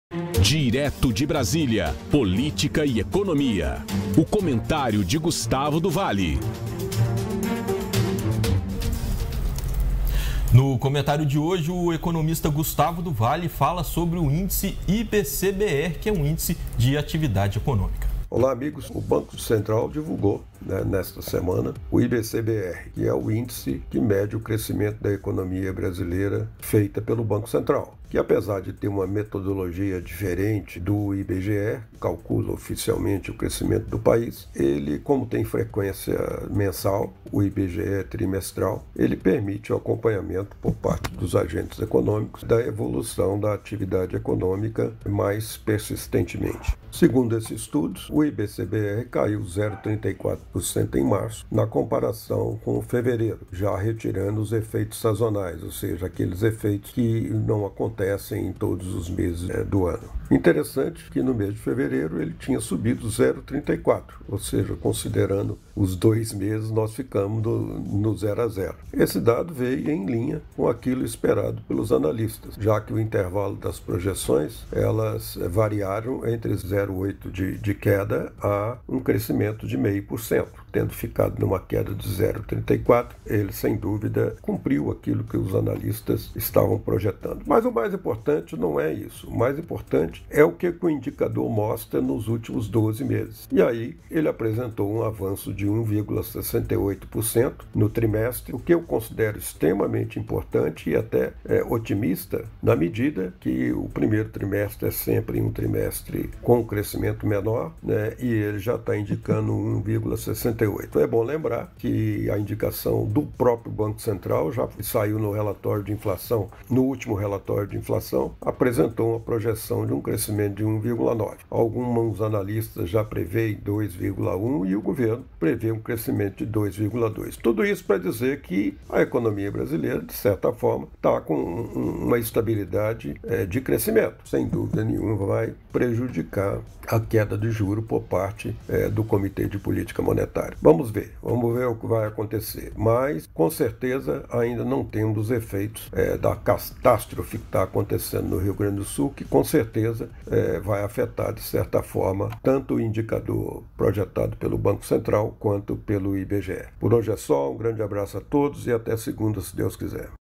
ECONOMISTA GUSTAVO DO VALE COMENTA SOBRE O IBC-BR -ÍNDICE DE ATIVIDADE ECONÔMICA
| BRASIL |

Direto de Brasília, o especialista em economia e política, Gustavo do Vale, comenta sobre a divulgação do IBC-BR e sobre a perspectiva econômica do Brasil. Acompanhe na íntegra: